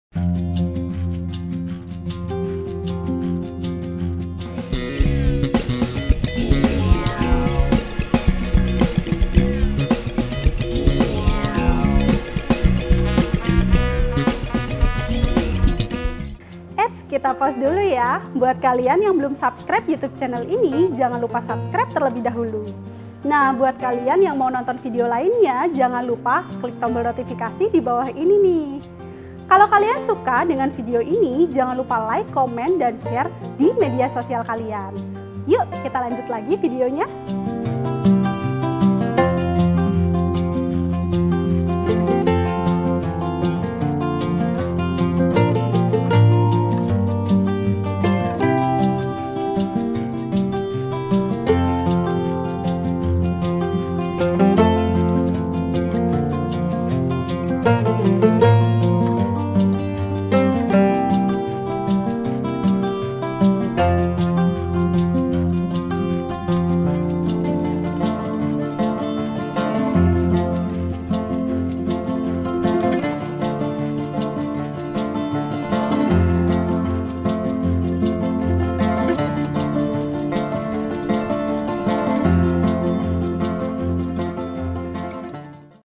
Resep Garlic Potato Cream Soup, Bisa Banget Buat Menu Sarapan Sebelum Mulai Beraktifitas!
Produser: Intan Yusan Septiani
Penulis Naskah: Indri & Ajeng
Peraga: Resi, Indri, Clara, Qorin
Fotografer: Andrian, Osyad, Bram
Videografer: Andrian, Osyad, Bram
Video Editor: Andrian, Osyad, Bram